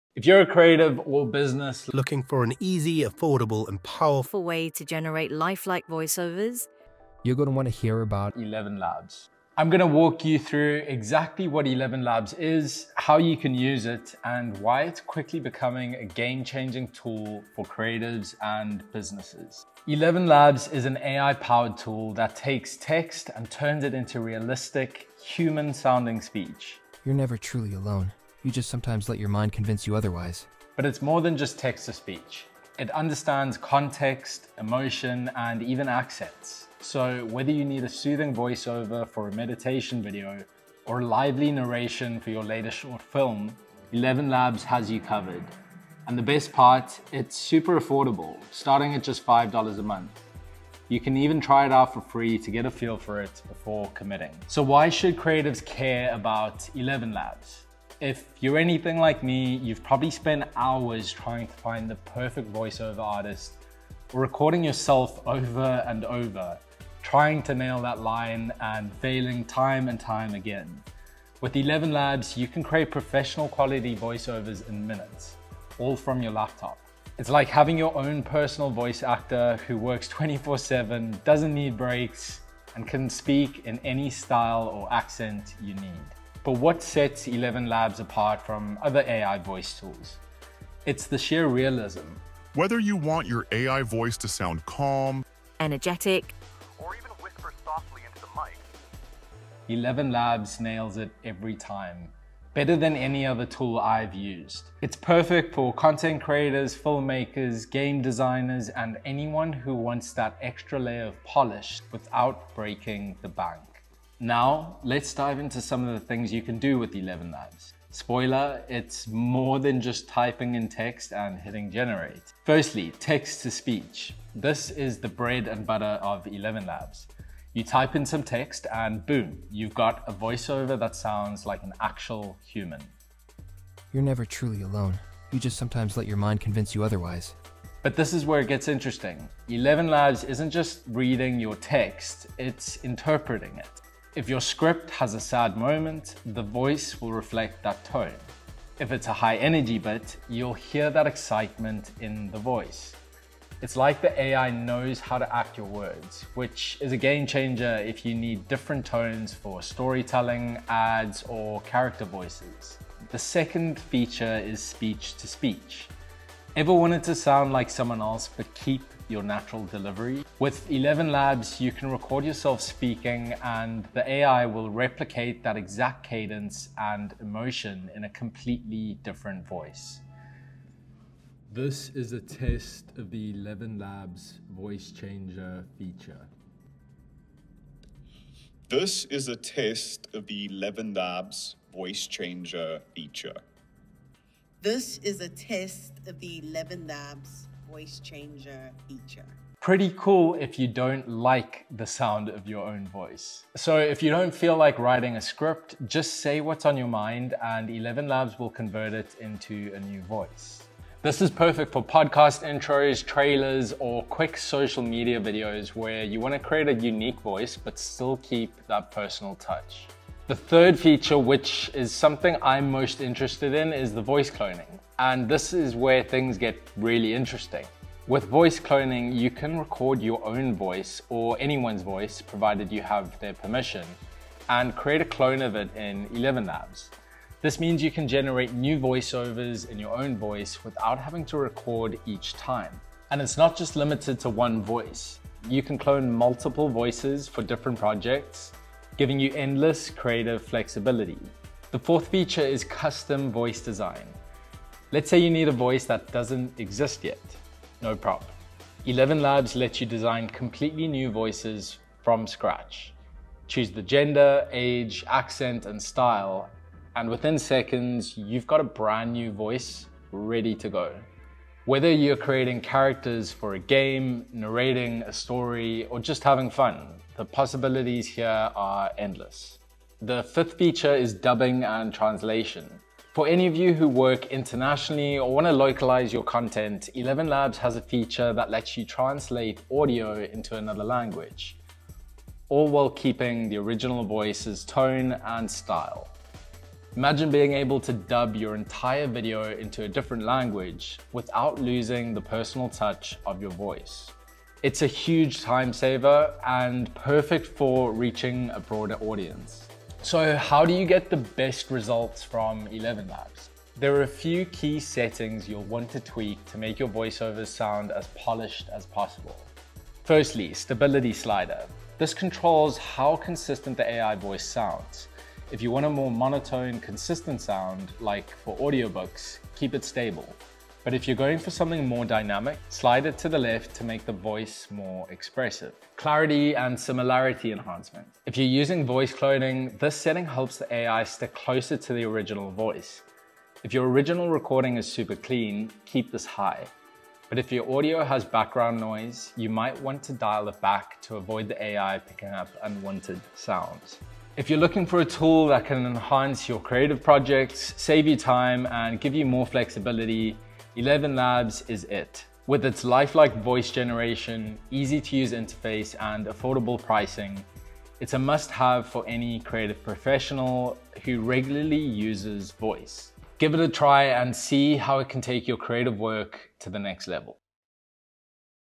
Eleven Labs Tutorial: AI Voice Generator, AI Text-to-Speech, Voice Cloning & AI Voice Design ✨
Eleven Labs Tutorial: AI Voice Generator, AI Text-to-Speech, Voice Cloning & AI Voice Design ✨ Unlock the full potential of Eleven Labs, the cutting-edge AI voice tool that’s changing the game for content creators and businesses alike.

In this in-depth tutorial, you’ll learn how to use Eleven Labs for everything from text-to-speech to speech-to-speech, as well as explore advanced features like voice cloning and custom voice design.

Whether you’re a YouTuber, podcaster, filmmaker, or game designer, Eleven Labs offers lifelike, human-sounding voiceovers that can elevate your creative projects to new heights.

We’ll guide you step by step through the AI voice generator process, showing you how to create professional-quality voiceovers in minutes. No need to spend hours recording or hiring voice actors when you can generate voices that match your script’s tone, style, and emotion with Eleven Labs’ powerful AI speech synthesis.

Whether you need a calm narration for a meditation video or a lively, energetic voice for your latest YouTube content, Eleven Labs delivers results that are perfect for creative AI tools.

Plus, we’ll cover the best practices for using custom voice design to craft unique voices tailored to your specific project, from character voices for indie games to branded voiceovers for marketing videos. You’ll also learn how voice cloning can save you time by replicating your own voice or the voice of others (with permission) to create new content without having to re-record every time.

In this tutorial, we’ll dive into how AI for creatives can speed up your workflow and help you scale your content production. Whether you’re making podcast voiceovers, narrating videos for YouTube, or developing characters for your next film or game, Eleven Labs offers endless possibilities for enhancing your creative output.

If you’re a creative professional looking to streamline your voiceover process, this AI voice tutorial is for you. Learn how to get the most out of Eleven Labs’ intuitive interface, tweak settings for more natural speech, and discover how to use the tool for a variety of applications, including podcast voiceovers, voice acting, and AI speech synthesis for different languages and accents.

By the end of this Eleven Labs tutorial, you’ll see why this is a must-have AI tool for anyone serious about improving their content with AI voice generation. Whether you’re producing professional videos, creating engaging podcasts, or building a standout indie game, Eleven Labs can take your projects to the next level.